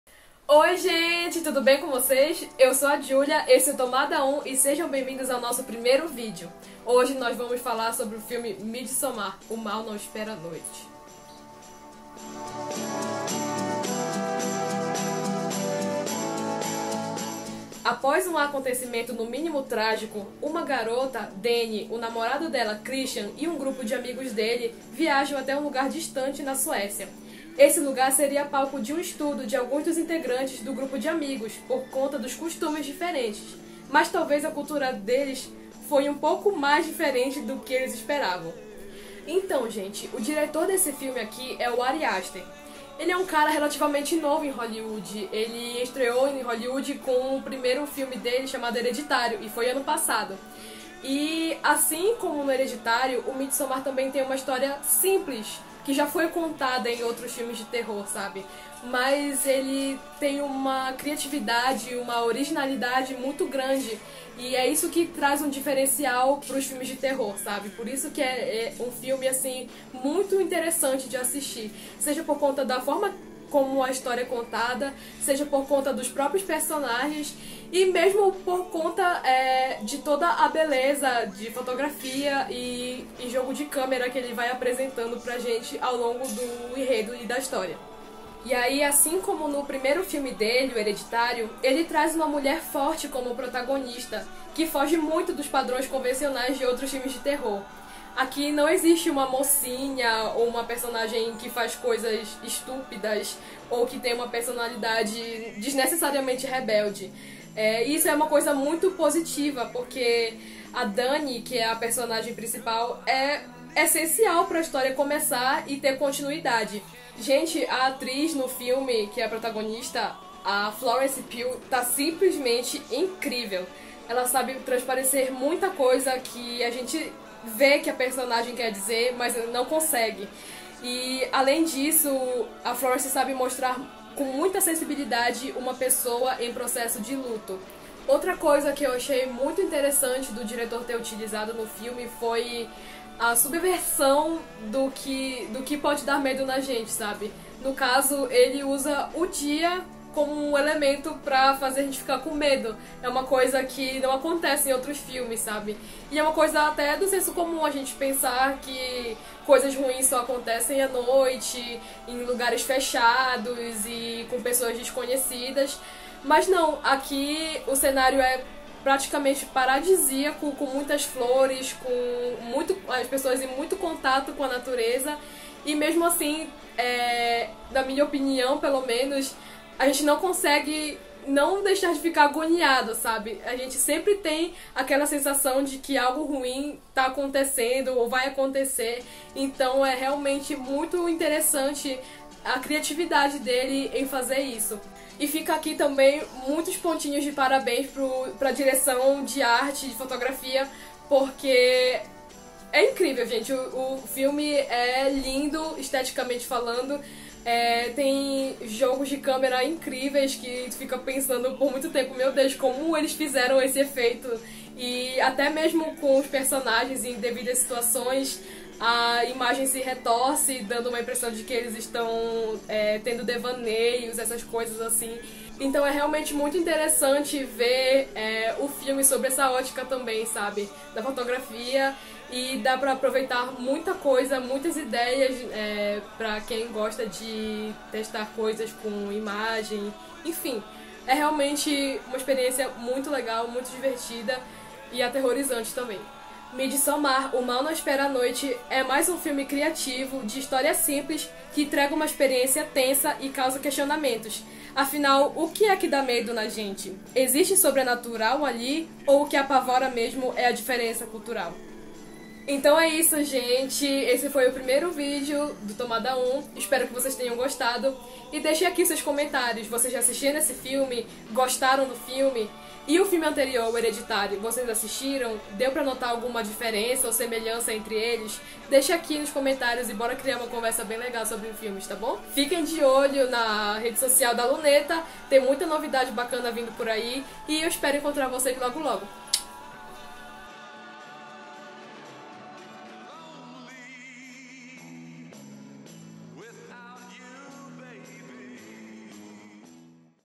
MIDSOMMAR - O MAL NÃO ESPERA A NOITE | TOMADA UM
Olá, maninhas e maninhos!
Nesse primeiro vídeo, vamos conversar sobre "Midsommar - O mal não espera a noite", o novo filme de terror do diretor Ari Aster.

Ah, mas antes que tu se esqueça, vai lá no nosso instagram:
Nós somos uma nova produtora de conteúdo de Belém e queremos te convidar pra participar disso junto com a gente!

Créditos:
Gravação e Edição: Giullia Moreira.

Ficha técnica do filme:
Título original: Midsommar
Ano: 2019
Direção: Ari Aster
Duração: 147 minutos
Gênero: Terror
Classificação: +18